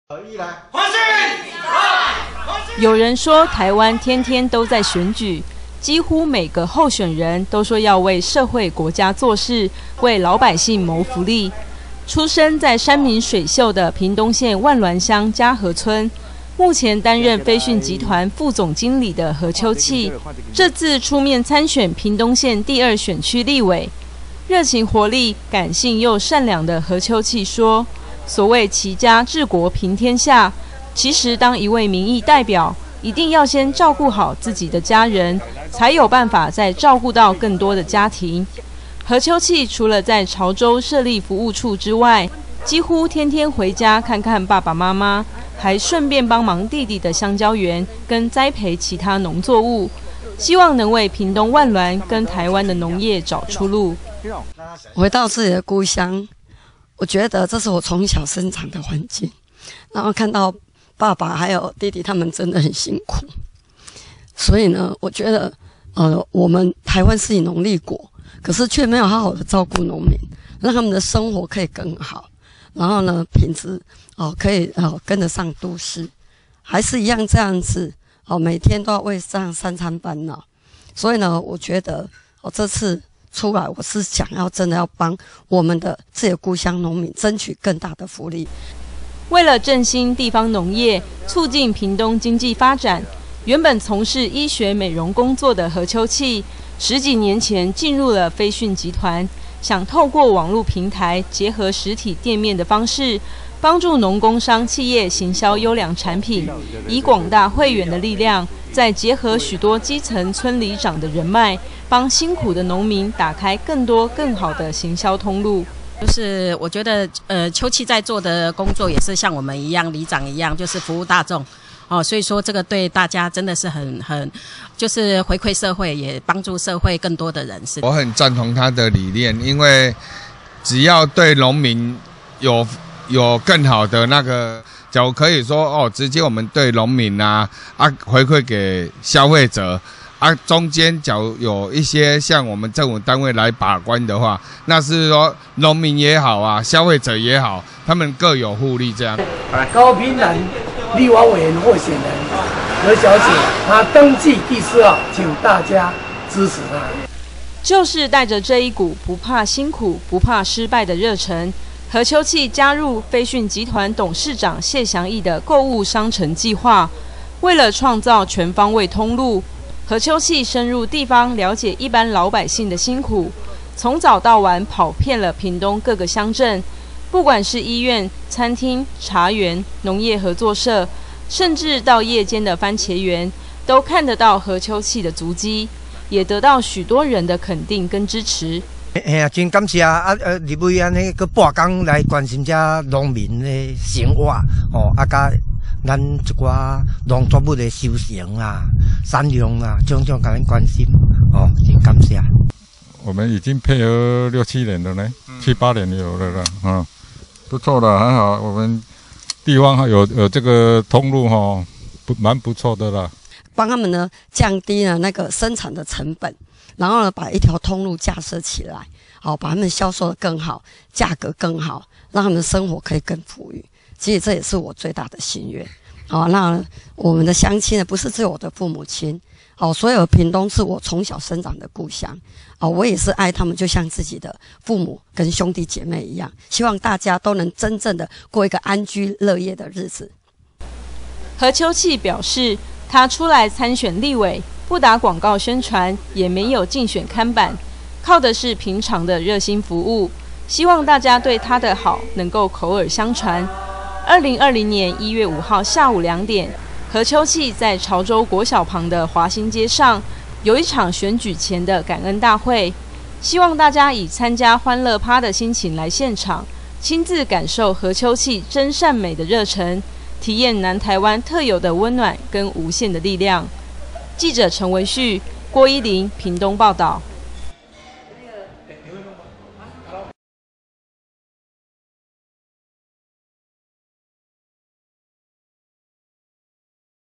《台傳媒新聞》20191223 屏東萬巒女兒何秋葺 立足潮州振興地方經濟
【記者 陳文旭、郭伊玲 / 屏東報導】
有人說台灣天天都在選舉，幾乎每個候選人都說要為社會國家做事、為老百姓謀福利，出生在山明水秀的屏東縣萬巒鄉佳和村，目前擔任飛訊集團副總經理的何秋葺，這次出面參選屏東縣第二選區立委，熱情活力、感性又善良的何秋葺說，所謂齊家治國平天下，其實當一位民意代表，一定要先照顧好自己的家人，才有辦法再照顧到更多的家庭，何秋葺除了在潮州設立服務處之外，幾乎天天回家看看爸爸媽媽，還順便幫忙弟弟的香蕉園跟栽培其他農作物，希望能為屏東萬巒跟台灣的農業找出路。
為了振興地方農業、促進屏東經濟發展，原本從事醫學美容工作的何秋葺，幾年前進入了飛訊集團，想透過網路平台結合實體店面的方式，幫助農工商企業行銷優良產品，以廣大會員的力量，再結合許多基層村里長的人脈，幫辛苦的農民打開更多更好的行銷通路。
就是帶著這一股不怕辛苦、不怕失敗的熱忱，何秋葺加入飛訊集團董事長謝祥益的購物商城計畫，為了創造全方位通路，何秋葺深入地方了解一般老百姓的辛苦，從早到晚跑遍了屏東各個鄉鎮，不管是醫院、餐廳、茶園、農業合作社，甚至到夜間的番茄園，都看得到何秋葺的足跡，也得到許多人的肯定跟支持。
何秋葺表示，她出來參選立委，不打廣告宣傳、也沒有競選看板，靠的是平常的熱心服務，希望大家對她的好能夠口耳相傳，2020年1月5號下午兩點，何秋葺在潮州國小旁的華興街上，有一場選舉前的感恩大會，希望大家以參加歡樂趴的心情來現場，親自感受何秋葺真善美的熱忱，體驗南台灣特有的溫暖跟無限的力量。